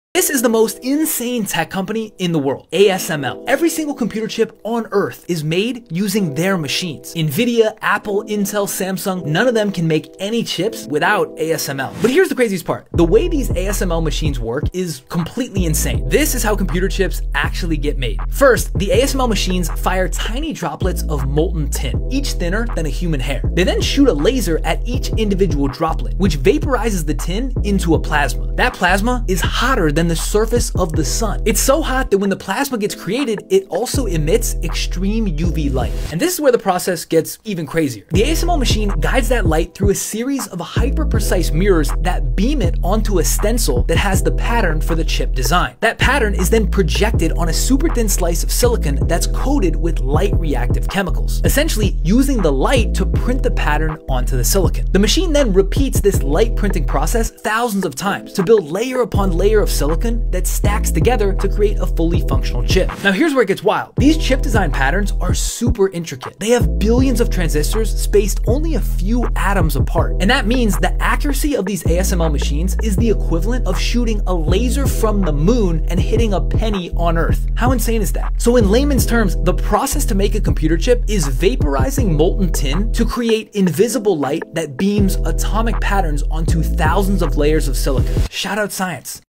Atomic-Scale Chipmaking: ASML + Apple’s Future of Tech
Every computer, phone, and device you use depends on chips. But did you know that only one company in the world can make the machines that print chips at the atomic level? That company is ASML. Using extreme ultraviolet lithography, ASML creates the technology that powers AI, smartphones, and the future of computing.

Even Apple’s latest products rely on chips made possible by ASML machines. Without them, there would be no iPhones, iPads, or Macs as we know them.

Get ready to see how chips are made on an atom scale — a true mind-bending process that most people don’t even know exists!